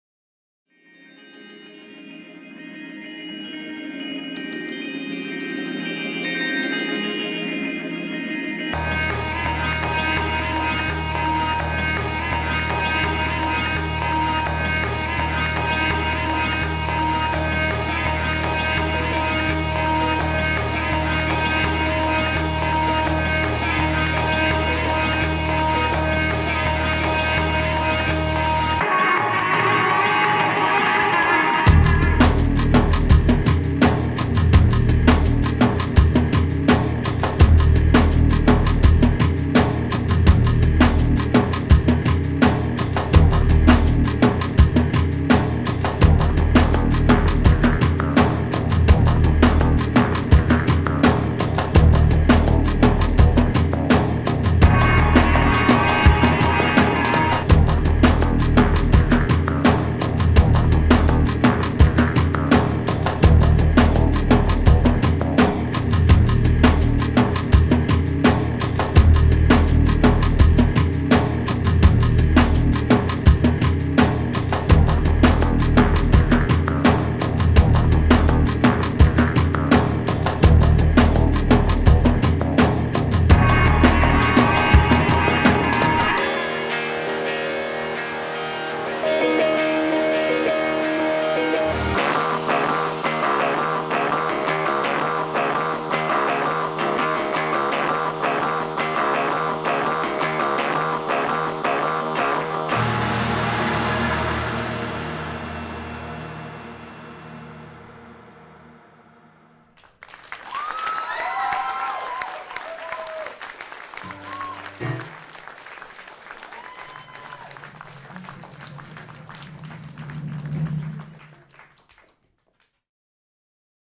Demo Reel 2006 - Nathaniel Walters / Little Red Dots
A short sample of some of the projects I worked on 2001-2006.